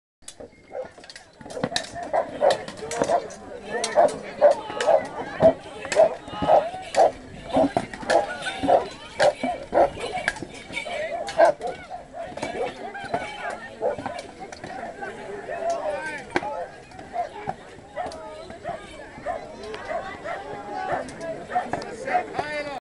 Stapelburger Mittelaltermarkt 2010
20 Schwerbewaffnete bei der Erstürmung der Burg,das gab ordentlich Krach beim aufeinander prallen.